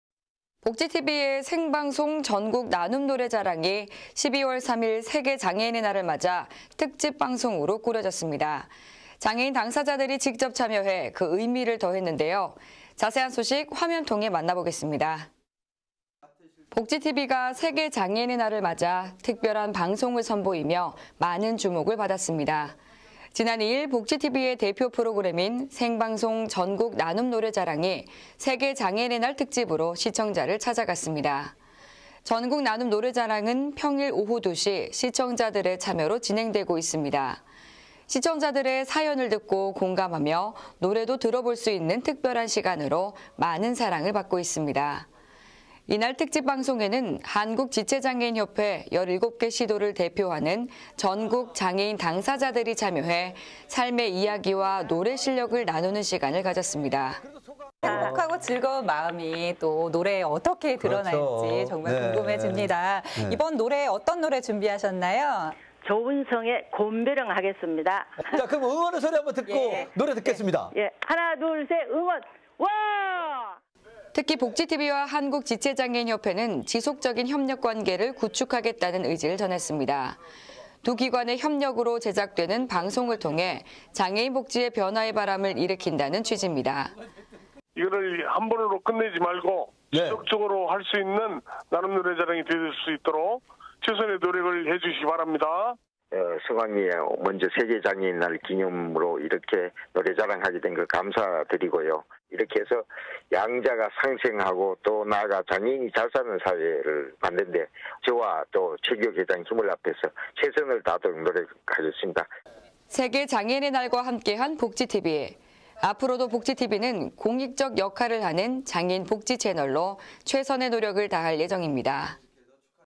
복지TV뉴스24 _ 전국나눔노래자랑, 한국지체장애인협회와 ‘특집 생방송’ / 2022.12. 7
아나운서 박정인/손자희